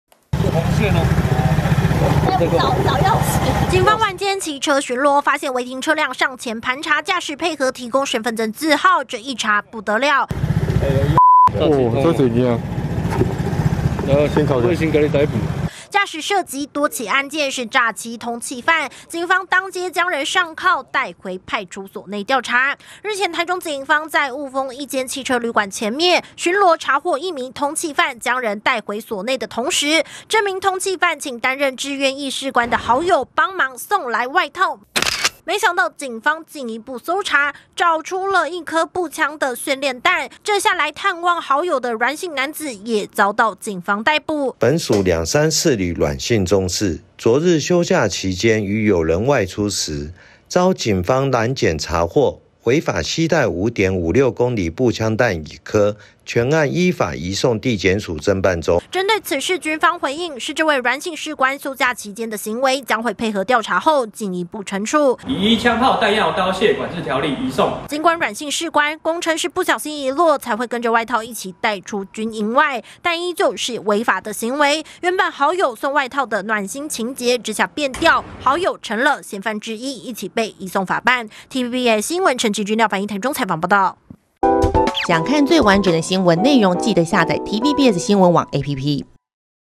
通緝犯找士官好友送外套 遭查獲藏「步槍彈」｜TVBS新聞
台中員警臨檢時，查獲一名王姓通緝犯，將人帶回派出所待查，同時這名通緝犯找來好友幫忙送外套給他穿，沒想到員警在檢查衣物時，發現一發軍用步槍訓練子彈，清查發現通緝犯找來的朋友，是國軍志願役士官，將他依槍砲彈藥管制條例移送法辦